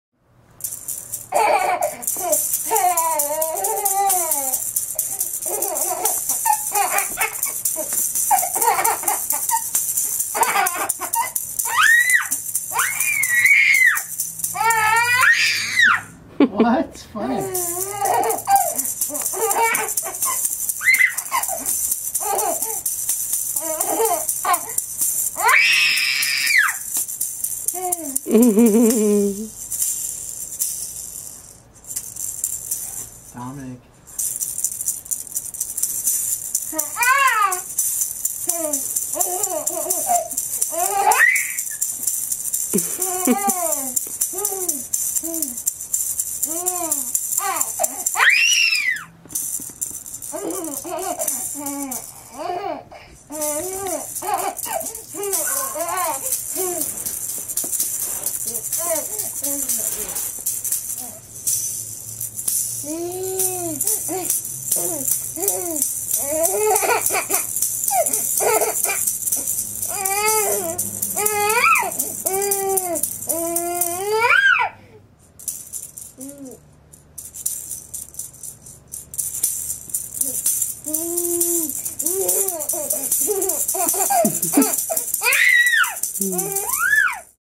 Dominic Laughing at a Rattle
Here's a quick video to brighten up a very sad day for me. Though the image quality lacks (next time we'll turn on some lights) Dominic's laughter makes up for it.

There is something about the simplicity of your child's laughter that is better than any drug could ever be.

Like usual we do this stuff for a few minutes and he laughs and laughs then we realize we should record it but by that time he's almost done laughing.

Tech note: I wanted to try out iMovie '09 to see if anything had improved, it still sucks so I'll stick to Vegas.